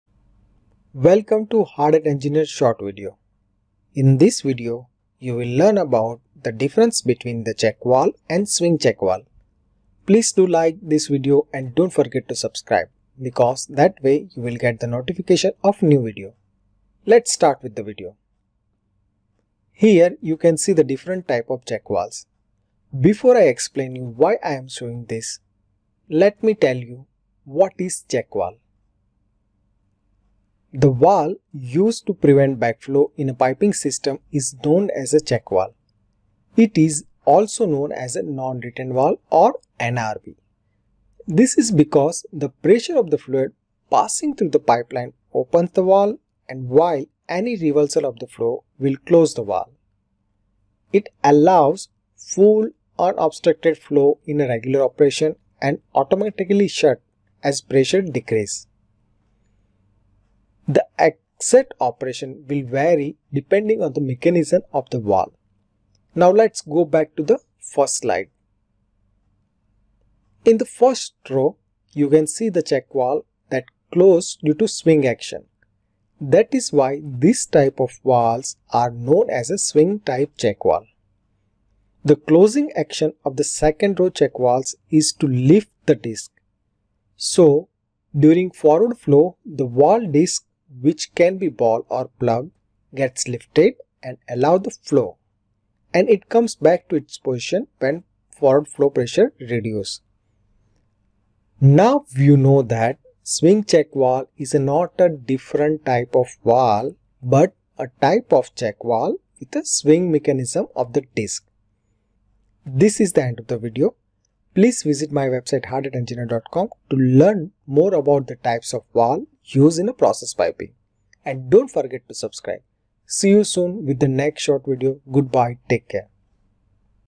What is the difference between check valve and swing check valve? [SHORT]
The valve used to prevent backflow in a piping system is known as a check valve. It is also known as a non-return valve or NRV.

00:00 Introduction
00:20 What is a Check Valve?
01:11 Lift Type and Swing Type Check Valve Mechanism